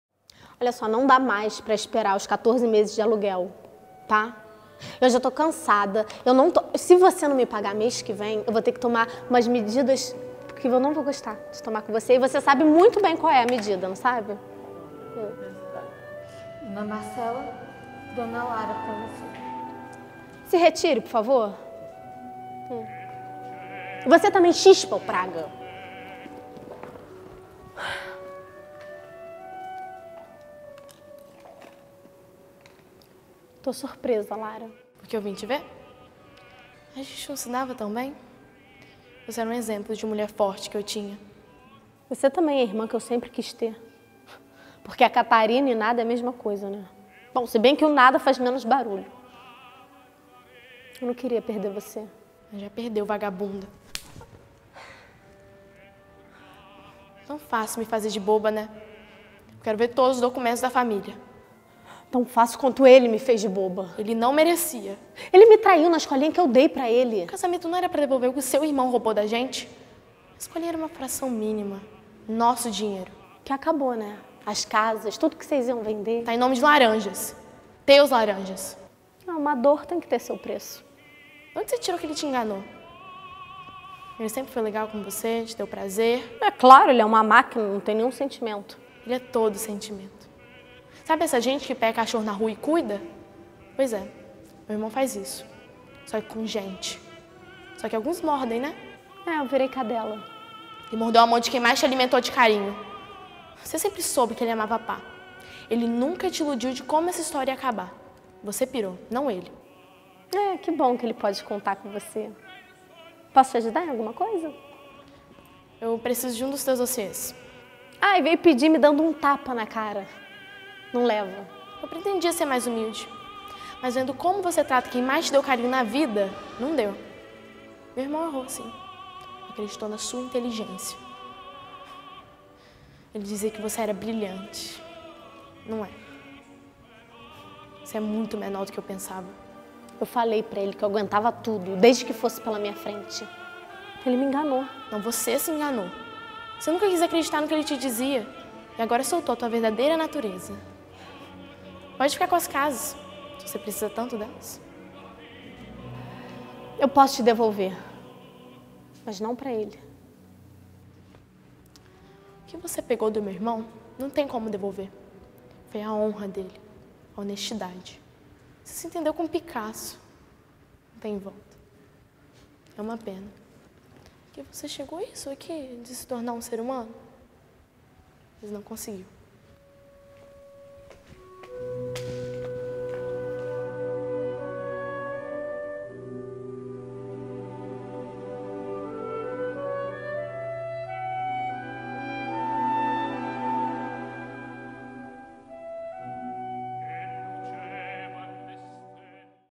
Lara Santana e Marcela Barrozo - Cena da novela Pecado Mortal
Cena gravada no Workshop de Interpretação para TV com Marcos Reis e gravação de cena com Marcela Barrozo.

Gravação realizada no dia 19/10/2014.

Agradecimento ao Clube Ítalo Brasileiro pelo apoio.